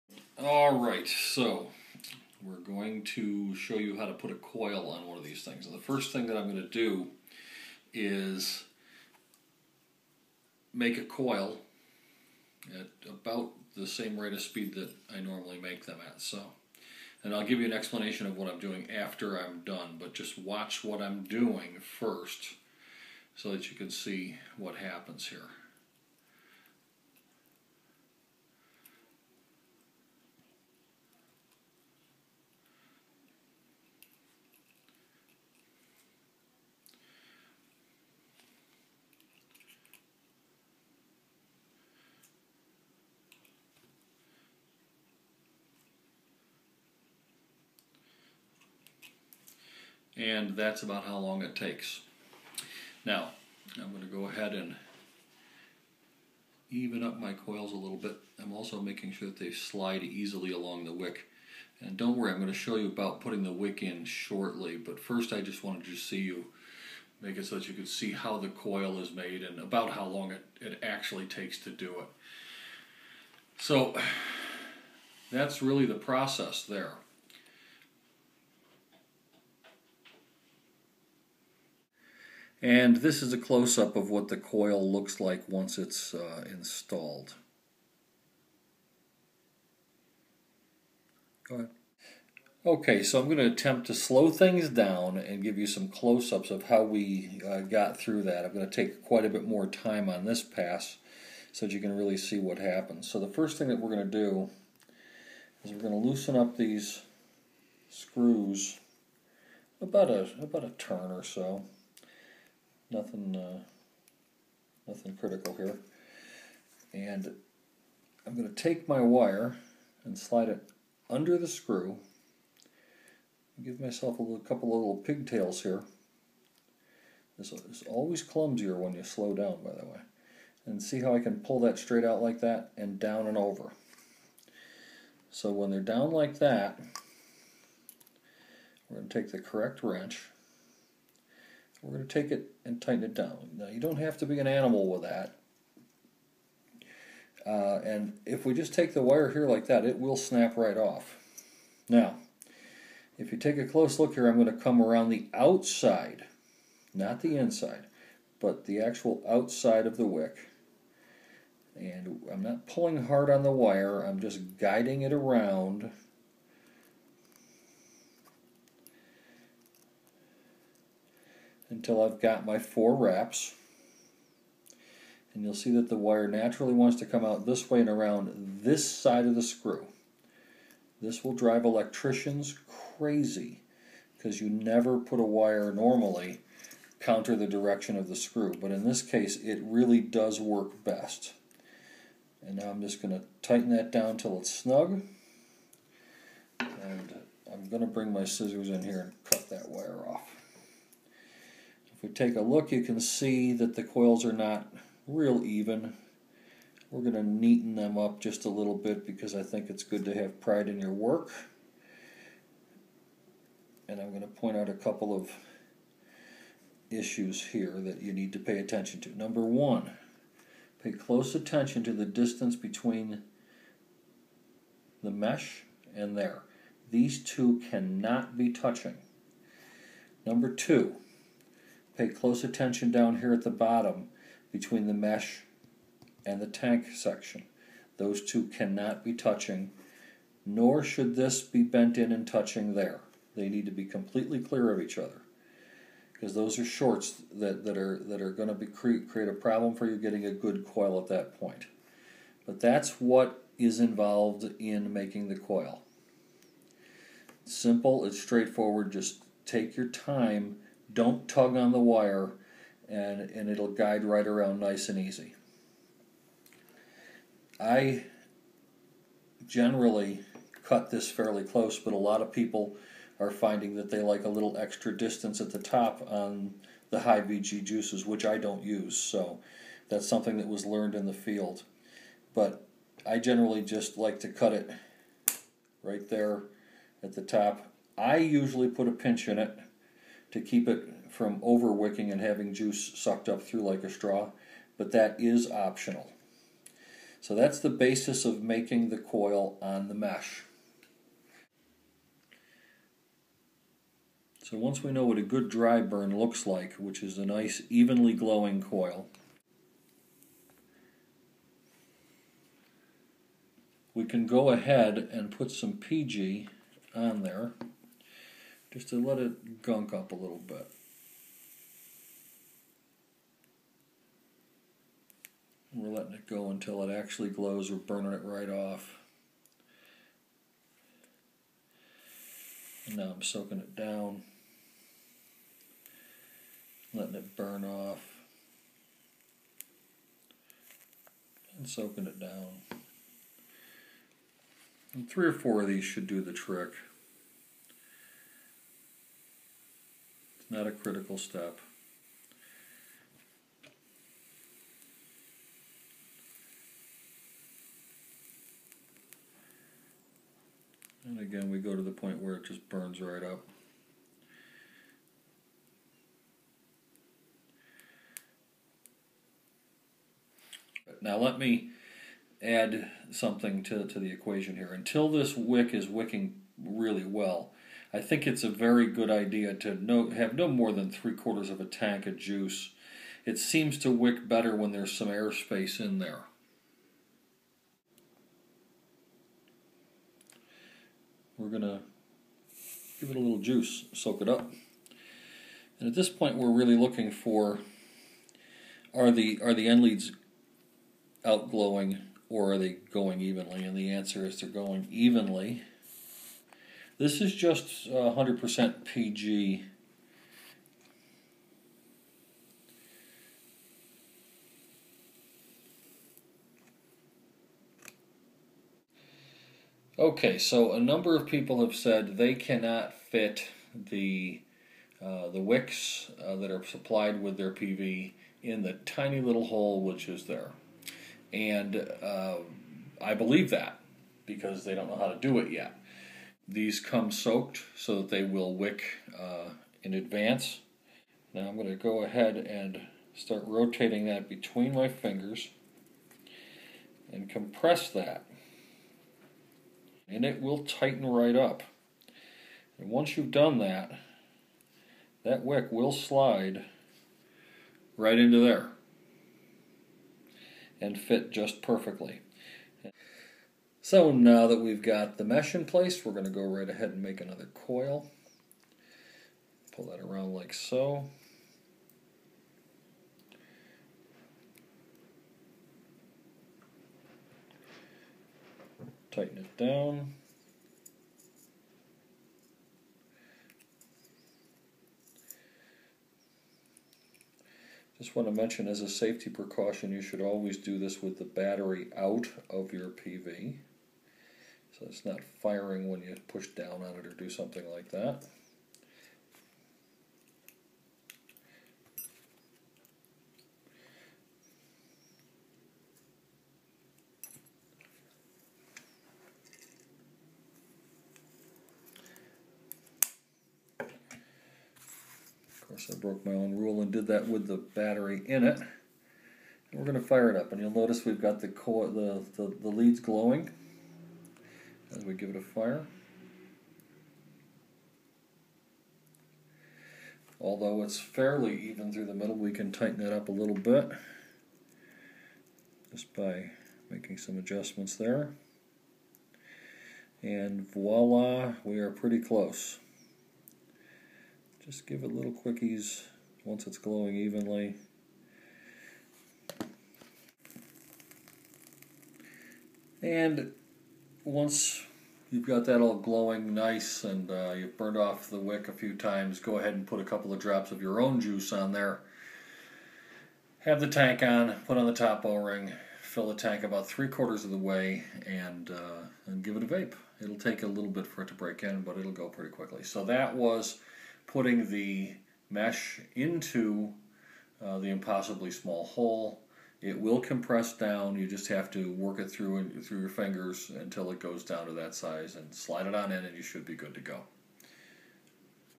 zenesis coil basics
The basics of making heater coils for the Zenesis PV